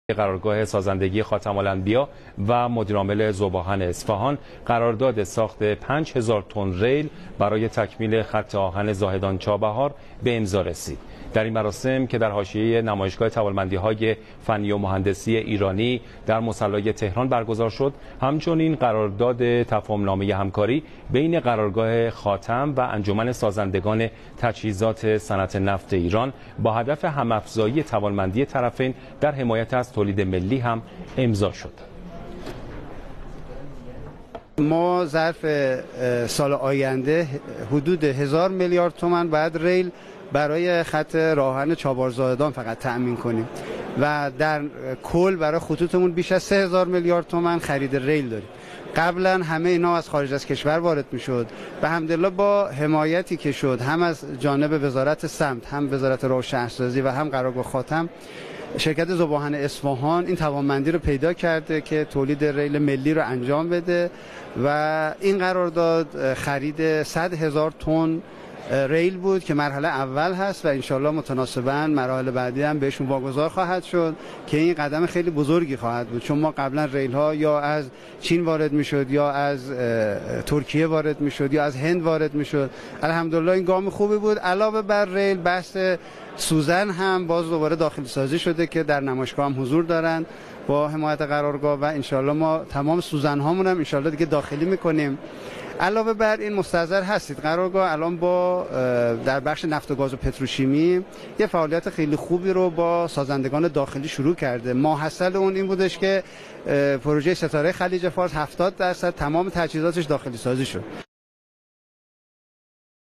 Iran Khatam base & Isfahan steel co. sign agreement for Zahedan Rail ساخت ريل راه آهن زاهدان
While Iran is moving toward full self-sufficiency in producing railroad tracks, foreign countries are offering attractive plans for the country’s railroad industry on a daily basis, Minister of Roads and Urban Development Mohammad Eslami محمد اسلامي said.

In comments at the opening of an exhibition of Iranian industrial achievements, Eslami said the country’s railroad industry is experiencing a major development in its infrastructures.

A new agreement on domestic production of railroad tracks -the financing for which would be supplied by the National Development Fund of Iran with the approval of Leader of the Islamic Revolution Ayatollah Seyed Ali Khamenei- is going to make Iran fully self-sufficient in manufacturing railroad tracks and obviate the need for imports once and for all, the minister added.

While the Islamic Republic has devised good plans to help the local production of locomotives and train cars thrive, foreigners are making attractive offers for the railroad track, train car and locomotive sectors on a daily basis despite the sanctions being still in place against Iran, Eslami added.

The minister went on to say that the parties that once formulated sanctions against Iran have now realized the great capacities inside the country and are attempting to undermine Iran’s path to self-sufficiency with attractive offers.

The Iranian administration has ramped up efforts in recent years to boost the country’s railroad industry.

Iran Khatam base & Isfahan steel co. sign agreement for 5,000 metric Ton Zahedan Rail production
قرارداد قرارگاه سازندگي خاتم الانبياء با ذوب آهن اصفهان ساخت ريل راه آهن زاهدان ايران